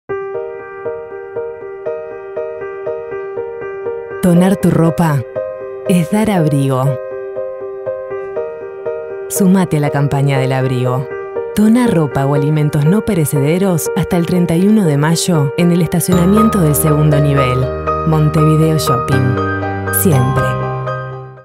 ¡Contamos contigo, para DarleLaVuelta!
Hasta el 31 de mayo podés donar ropa o alimentos no perecederos.
¡Te esperamos en Atención al Cliente en el estacionamiento del 2° nivel, de 10 a 18 h!